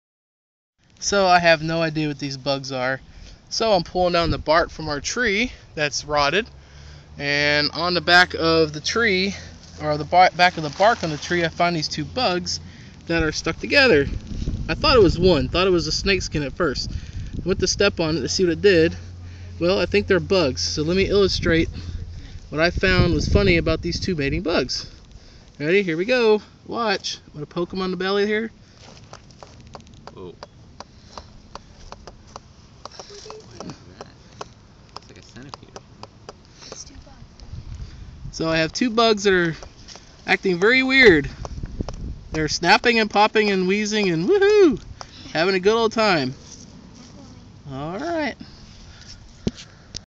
Eyed Elater
An "eyed elater" click beetle I found in my backyard.